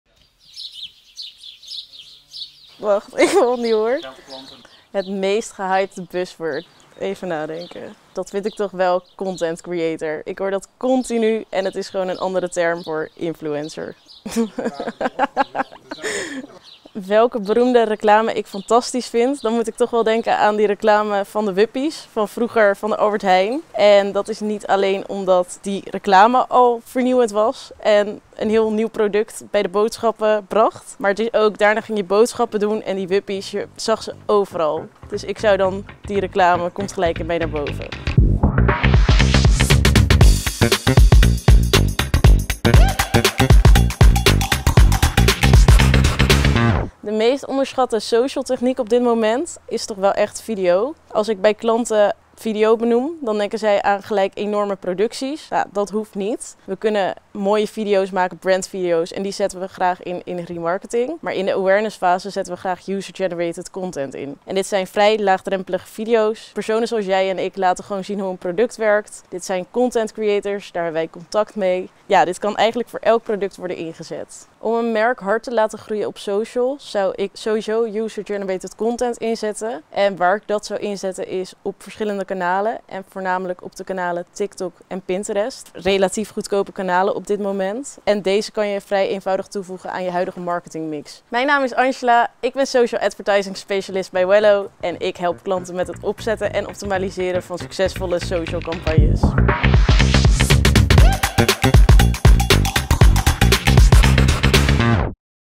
Waarom user-generated content onmisbaar is voor je marketing? Angela vertelt over UGC content.
We spraken Angela, social advertising specialist bij Whello, over social media marketing. Ze helpt merken met het opzetten en optimaliseren van succesvolle social campagnes.

Meer tips over user-generated video's van Angela? Lees haar blog